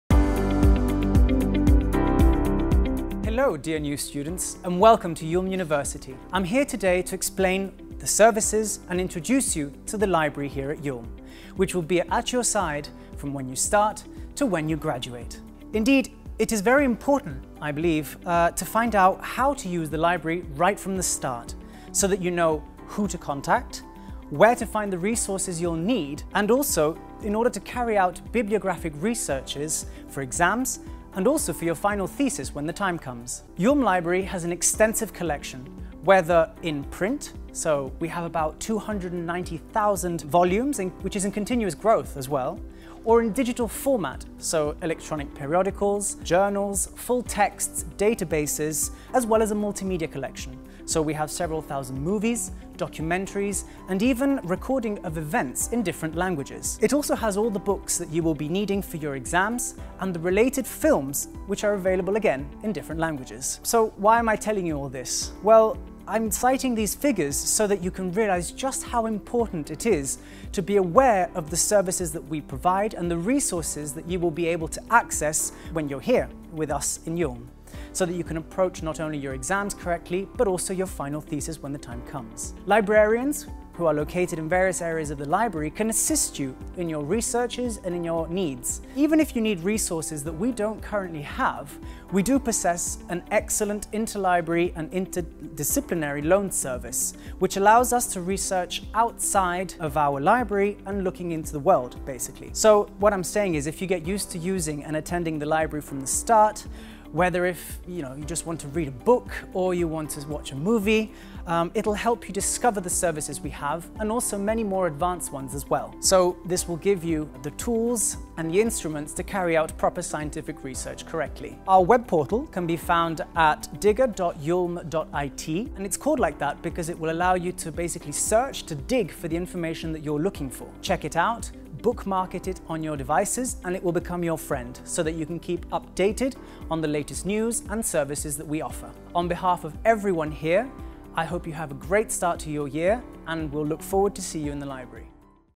Library
What are the services of the IULM Library? Find out in this video.